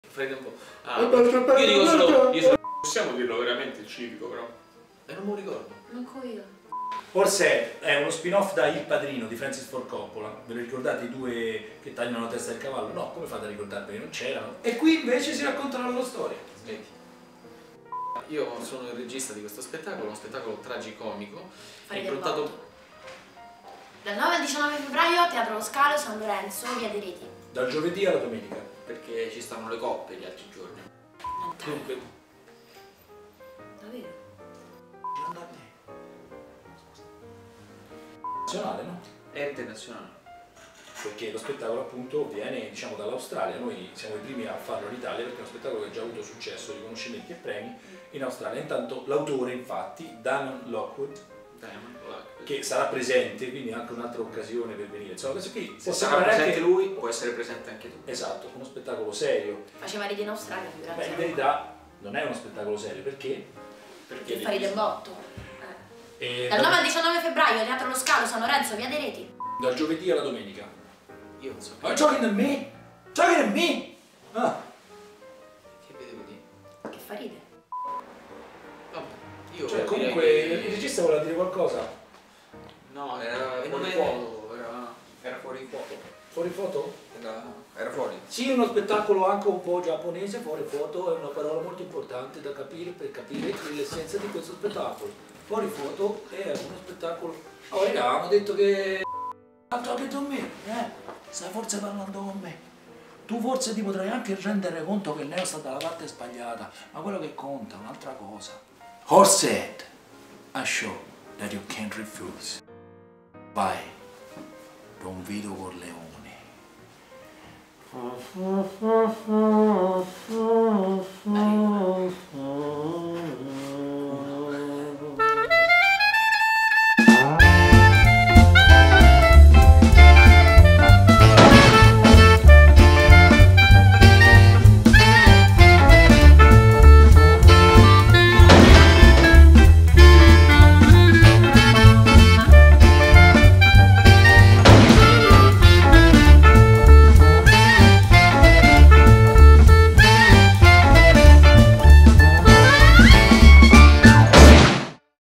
HorseHead Promo 3
Promo dello Spettacolo "HorseHead" di Damon Lockwood, Regia di Leonardo Buttaroni, con Diego Migeni e Sebastiano Gavasso
Dal 9 al 19 Febbraio 2012 Teatro allo Scalo Roma, via Dei Reti 36